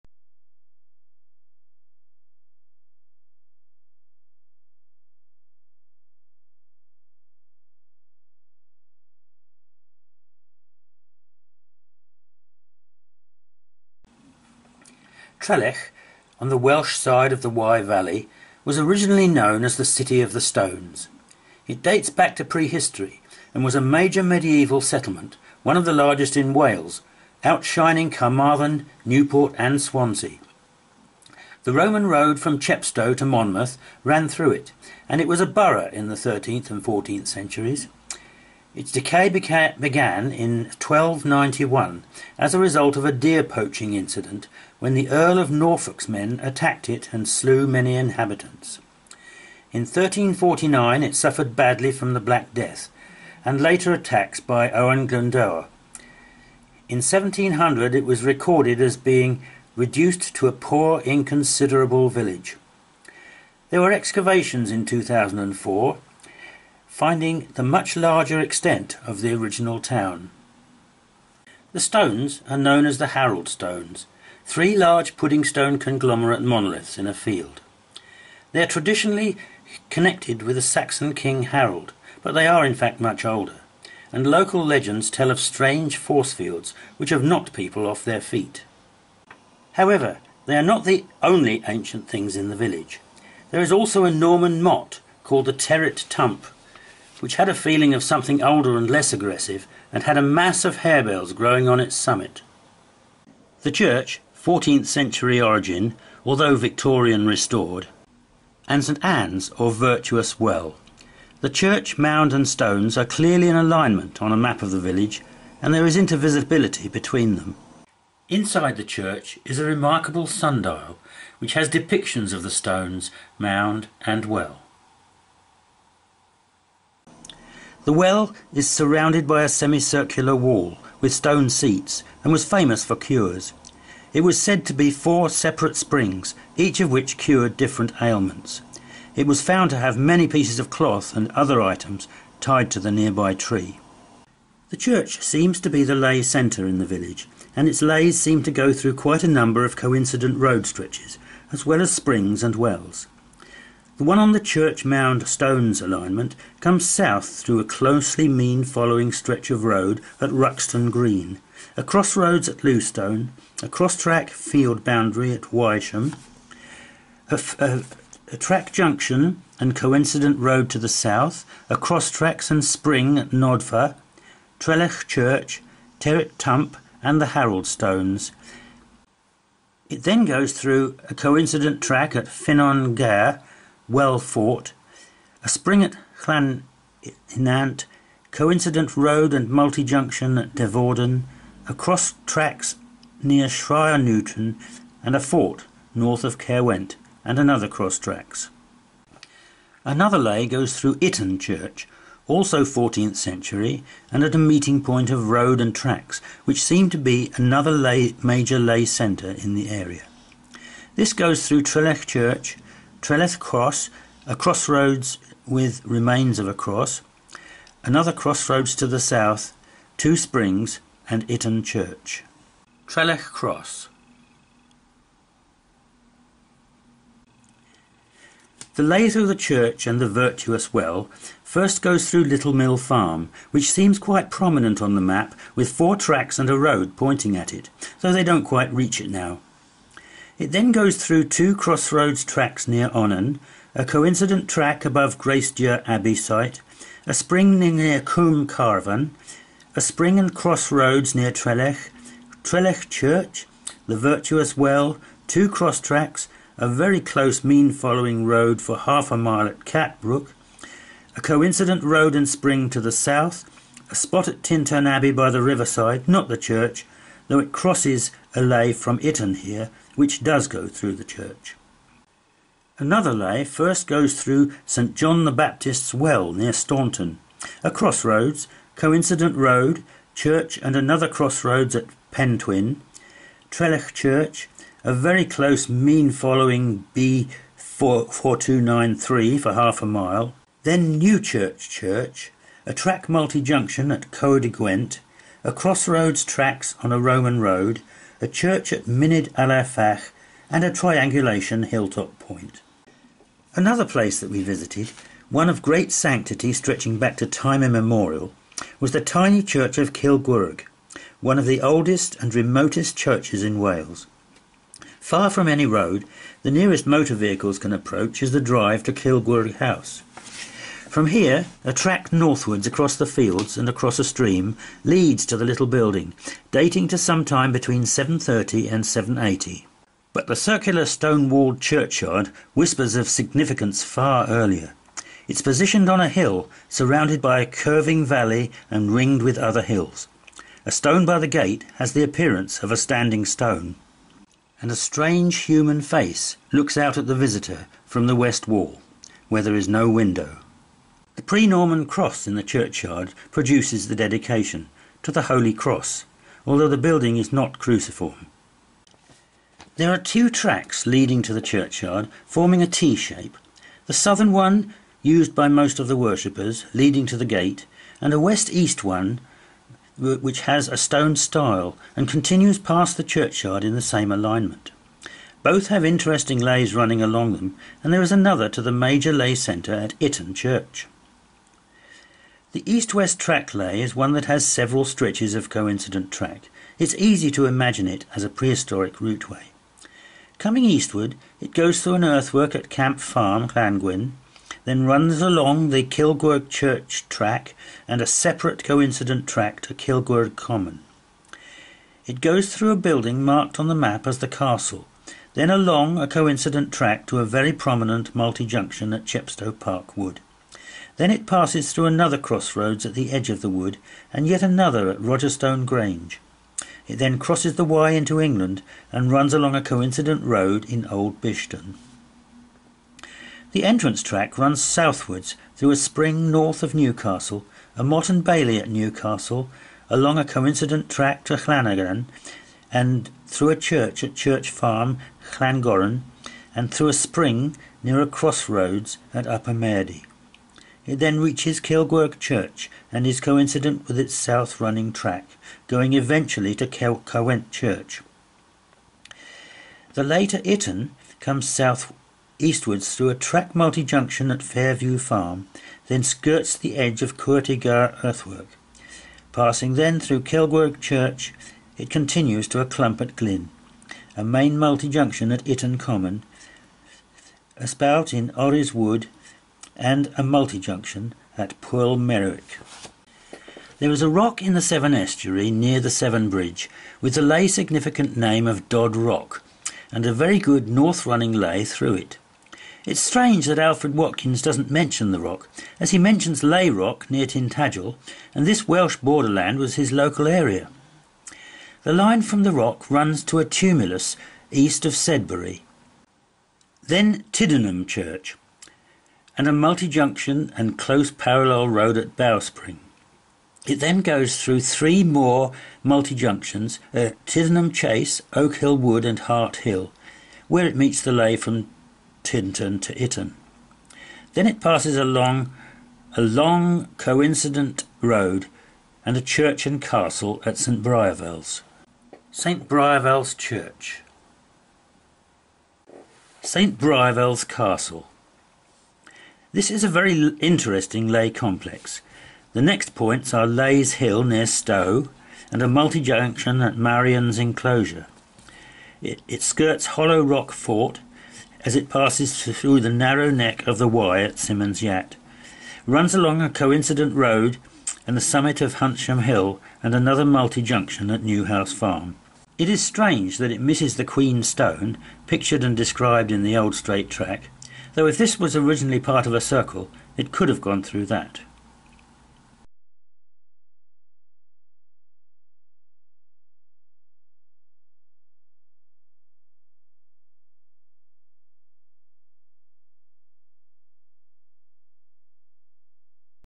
The City of the Stones, and Welsh Border Leys
Ley hunting on the border of Wales and England in 2004, beginning at Trellech, known as the City of the Stones, because of its once greater extent, and the monoliths known as the Harold Stones.  The stones, the church and a mound called the Terret Tump are in alignment on a good ley, and the church is a ley centre (meeting point of leys).  One of its leys goes through Itton Church, another ley centre which has on one of its alignments the ancient and remote church at Kilgwrrwg in its circular churchyard with possible monolithic stones in its wall, at a meeting point of tracks which are also leys.